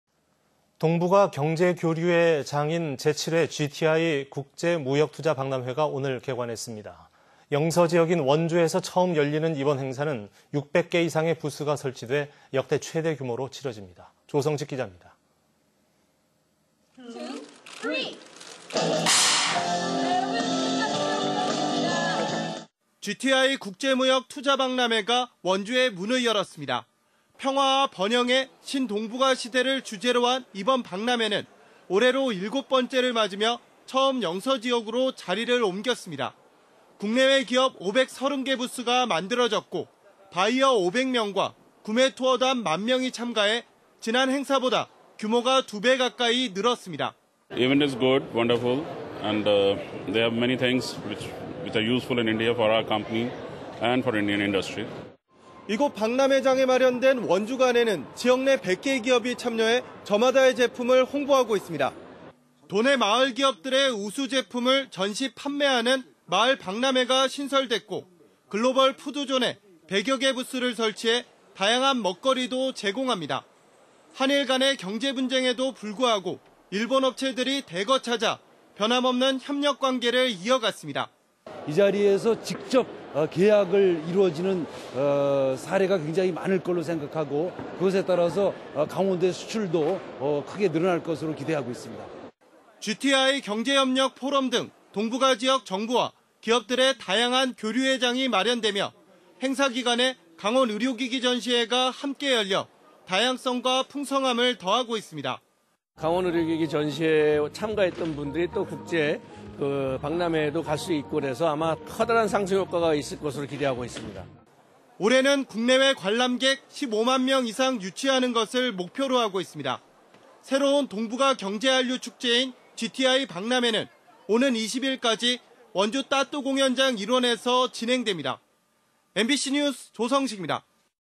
[뉴스리포트] 제7회 신동북아시대 GTI 국제무역/투자박람회 개관 20191017
동북아 경제 교류의 장인
제7회 GTI 국제무역.투자박람회가
오늘(17) 개관했습니다.

 영서지역인 원주에서 처음 열리는
이번 행사는 600개 이상의 부스가 설치돼
역대 최대 규모로 치러집니다.